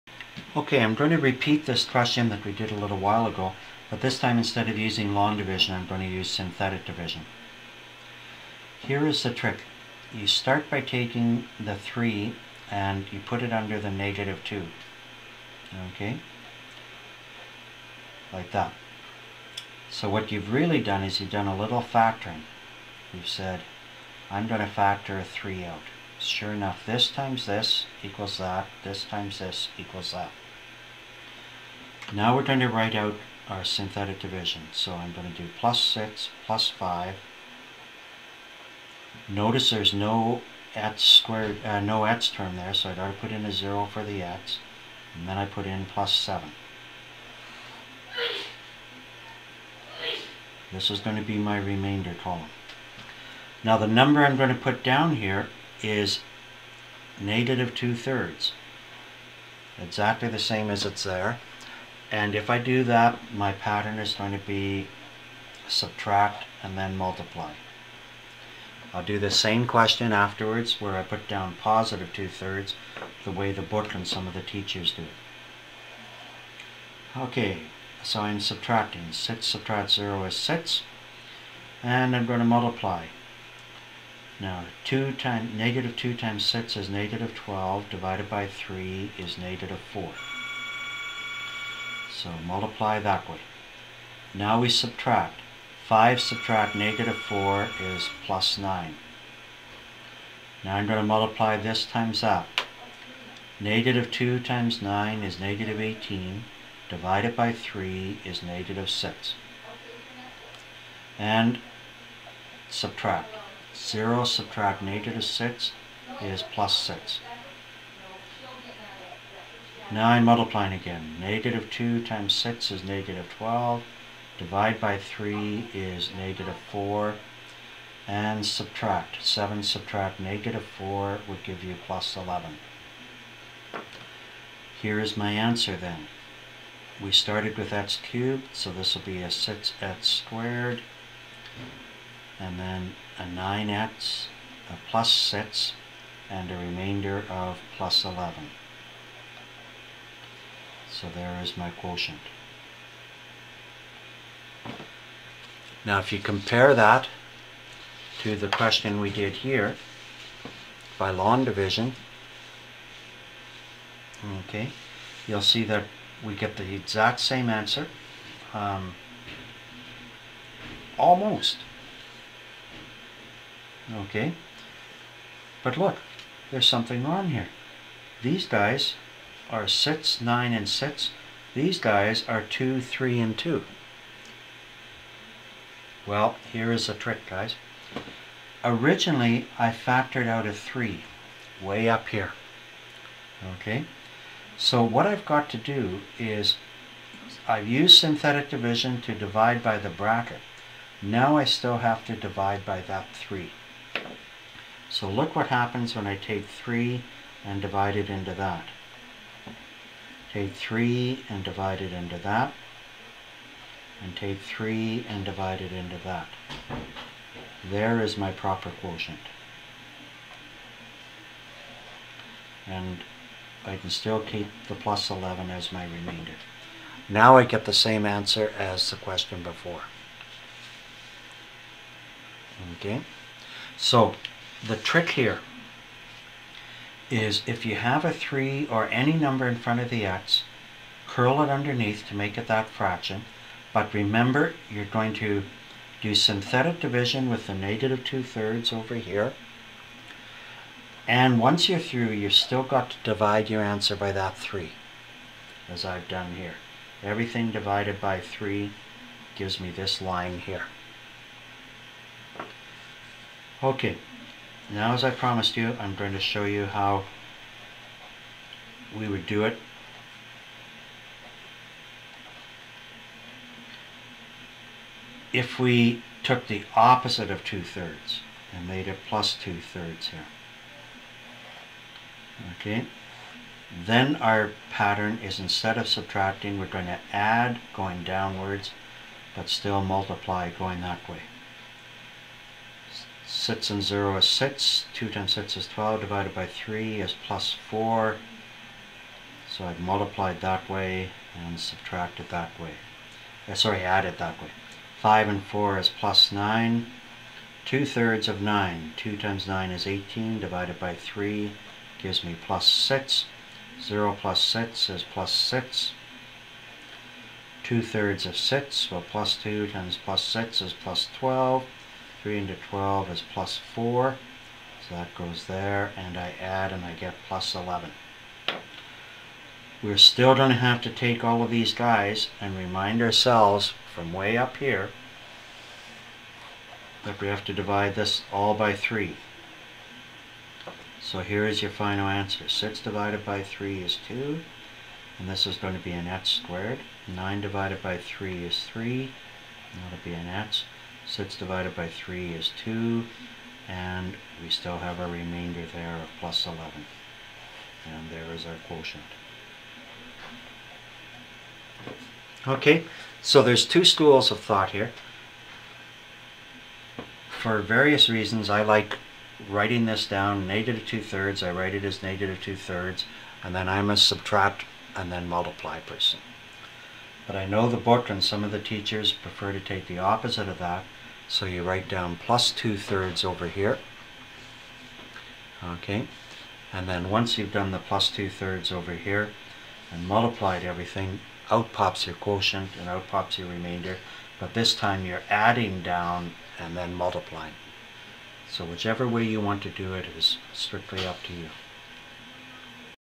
Synthetic Division part two.
Dividing a polynomial by a binomial using synthetic division (part two). This is video 28 of 51 in the solving equation series. The next three series are "Word Problems", "Factoring" and "Graphing".